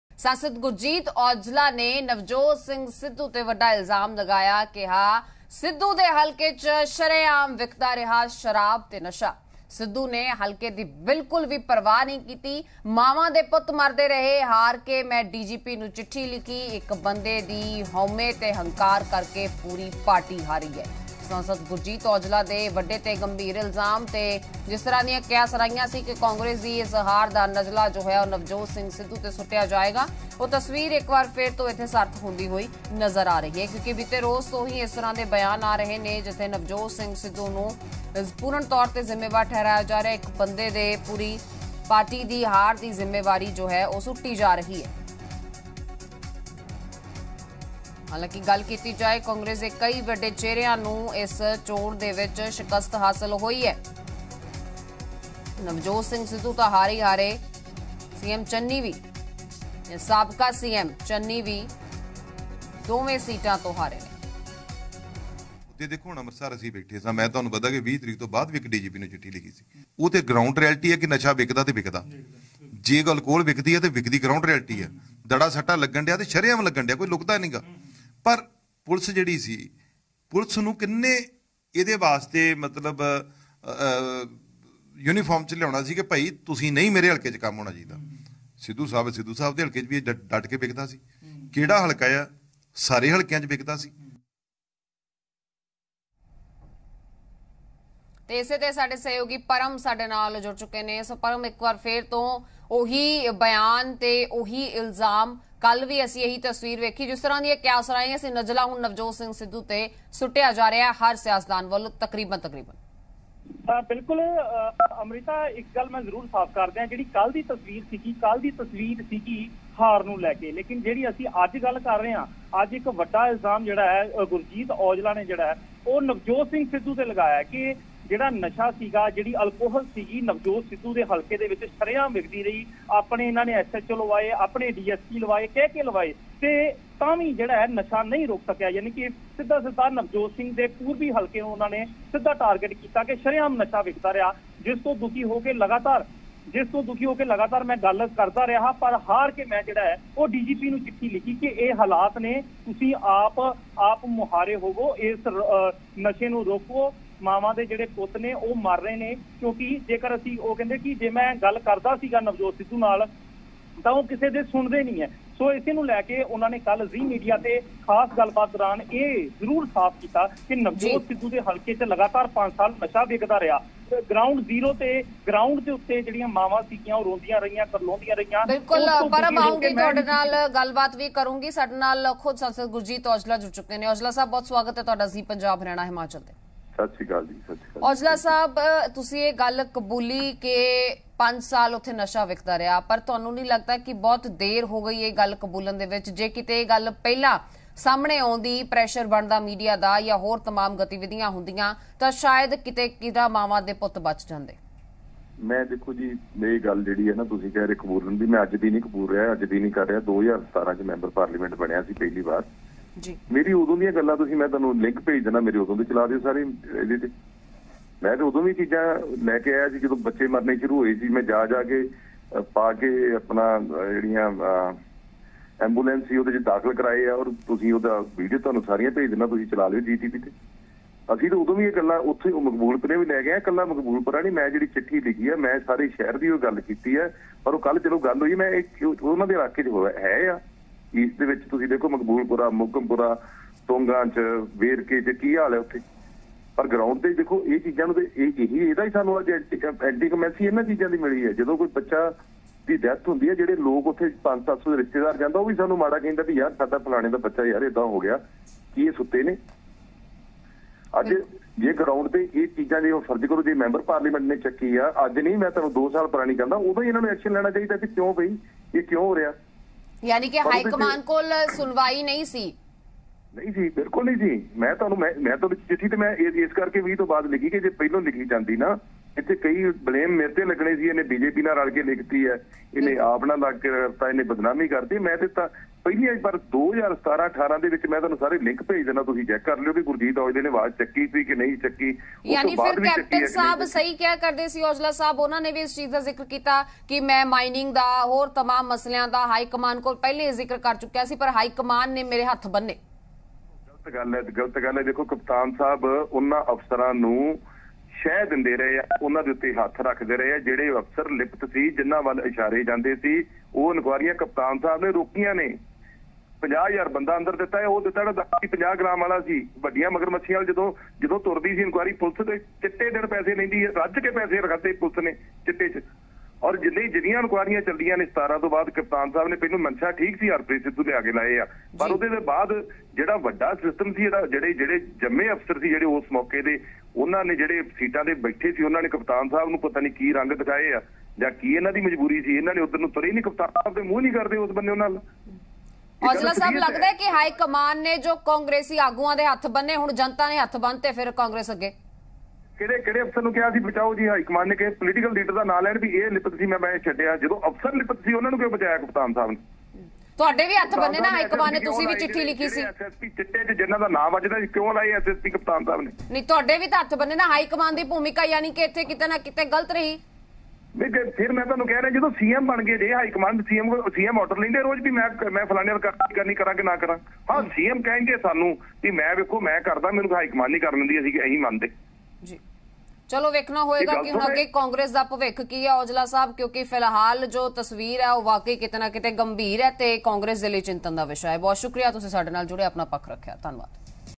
Navjot Singh Sidhu ਦੇ ਹਲਕੇ 'ਚ ਸ਼ਰੇਆਮ ਵਿਕਦਾ ਰਿਹਾ ਸ਼ਰਾਬ ਤੇ ਨਸ਼ਾ- Gurjeet Aujla | Punjab News | Zee PHH
Gurjeet Singh Aujla ਦਾ Navjot Singh Sidhu 'ਤੇ ਵੱਡਾ ਇਲਜ਼ਾਮ
'ਮਾਵਾਂ ਦੇ ਪੁੱਤ ਮਰਦੇ ਰਹੇ, ਸਿੱਧੂ ਦੇ ਹਲਕੇ ਵਿੱਚ ਸ਼ਰੇਆਮ ਵਿਕਦਾ ਰਿਹਾ ਸ਼ਰਾਬ ਤੇ ਨਸ਼ਾ'